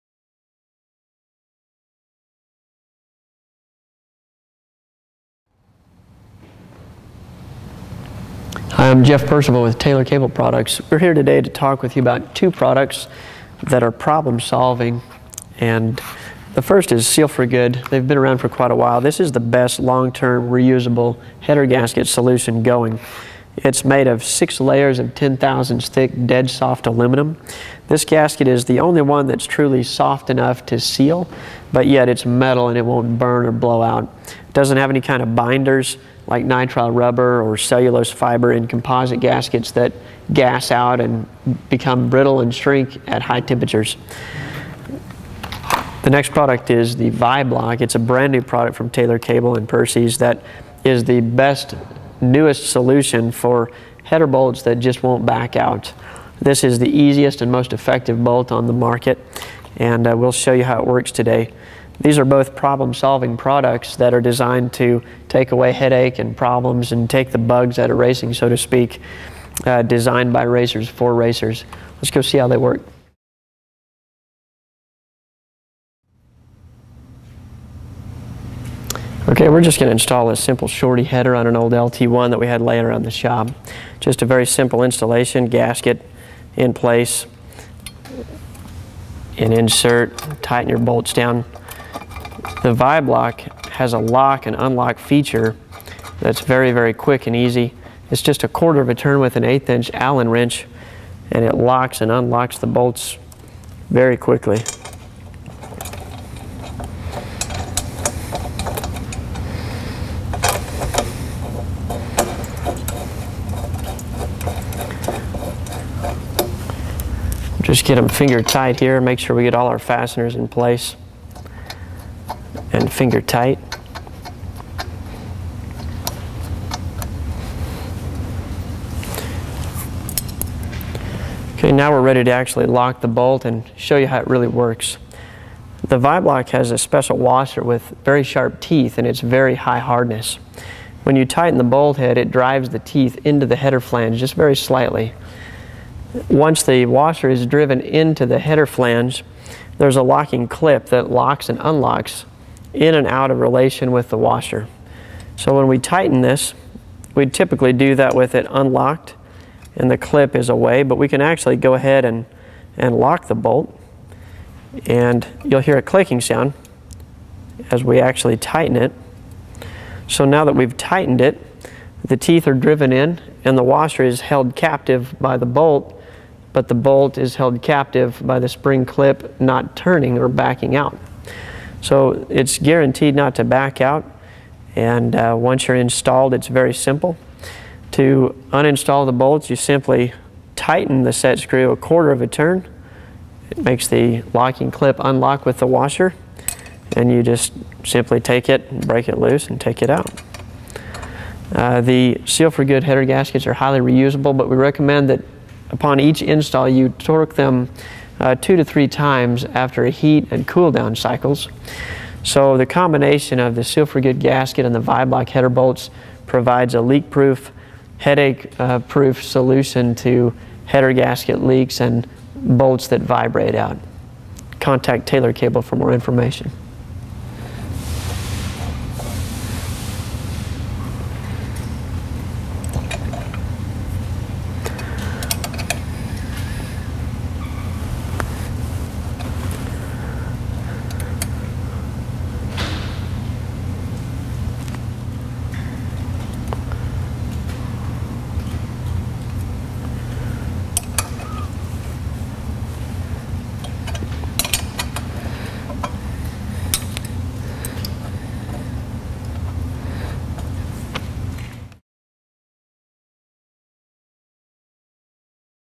Taylor Cable's Vibe-Lock - Vibration Proof Lock Fasteners
The Vibe-Lock TM Friction Lock Fastener design uses a locking set pin built in to the bolt, When tightened the set pin expands the bolt threads against the thread wall providing an unmatched lock. The Vibe-Lock TM Friction Lock Fastener is the only fastener that provides a locking system without using any additional wires, locking compounds, lock washers or complicated interrupters.